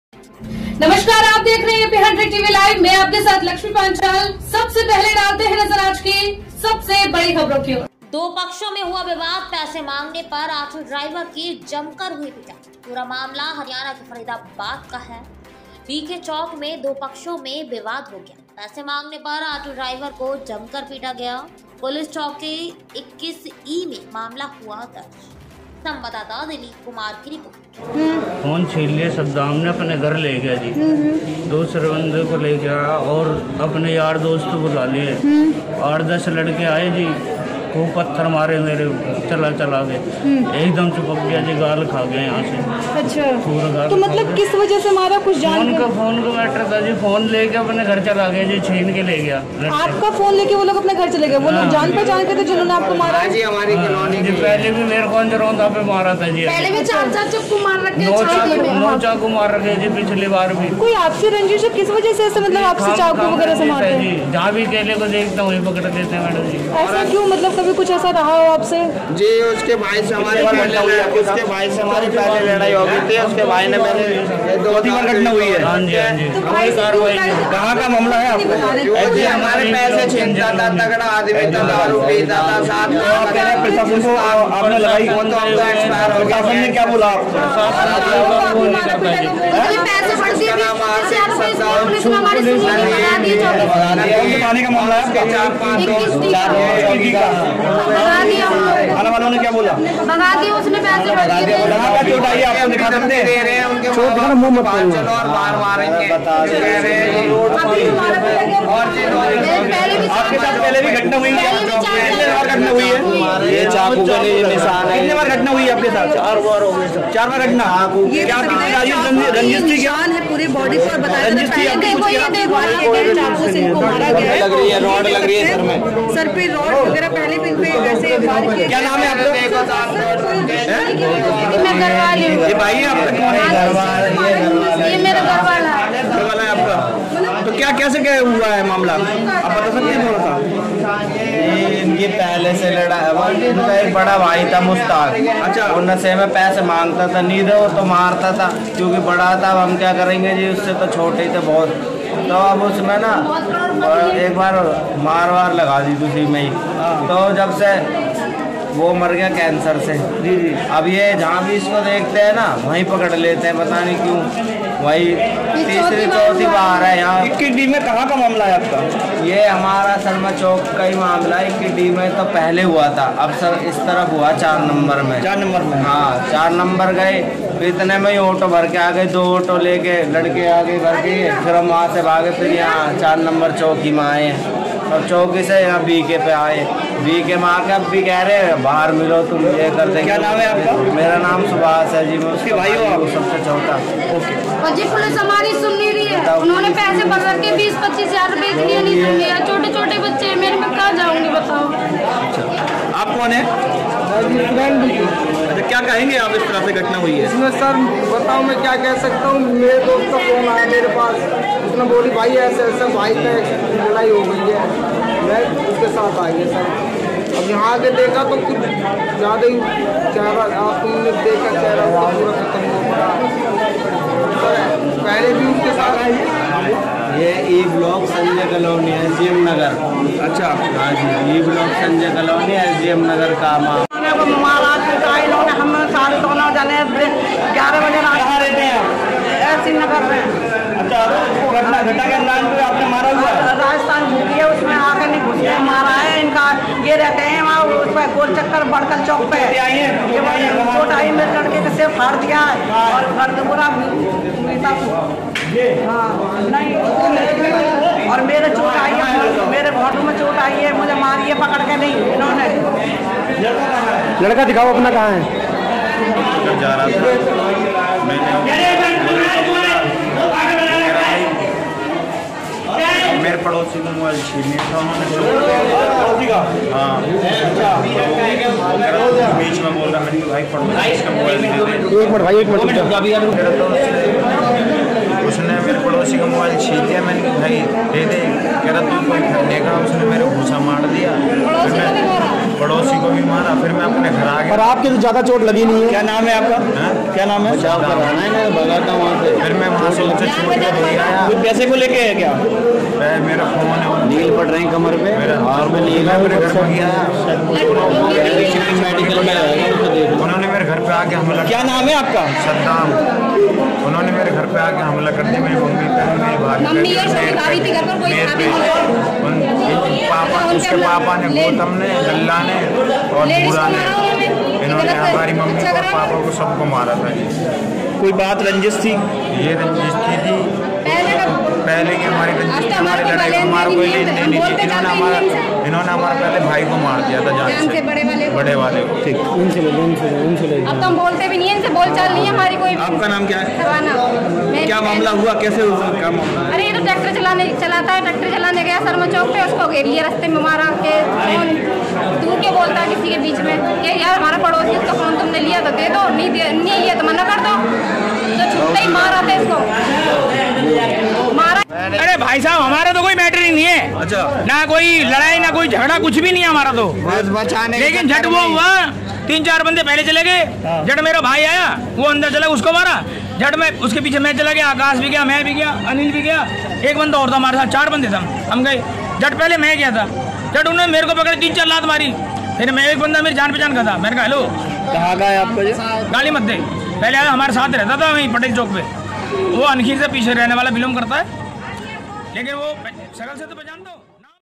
दो पक्षों में हुआ विवाद पैसे मांगने पर ऑटो ड्राइवर की जमकर हुई पिटाई
हरियाणा के फरीदाबाद में बीके चौक में दो पक्षों में विवाद हुआ पैसे मांगने पर ऑटो ड्राइवर को जमकर पीटा गया पुलिस चौकी 21E में मामला हुआ दर्ज|

 संवाददाता दिलीप कुमार की रिपोर्ट